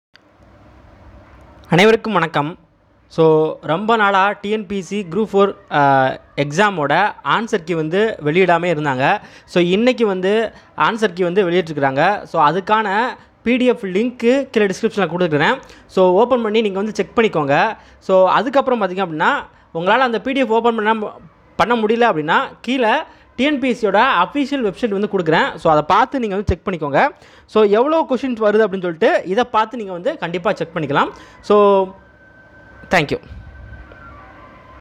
Original Tnpsc Group 4 Answer key 2019 | Official Group 4 answer key released
At last TNPSC Published CCSE-IV GROUP-4 Keys.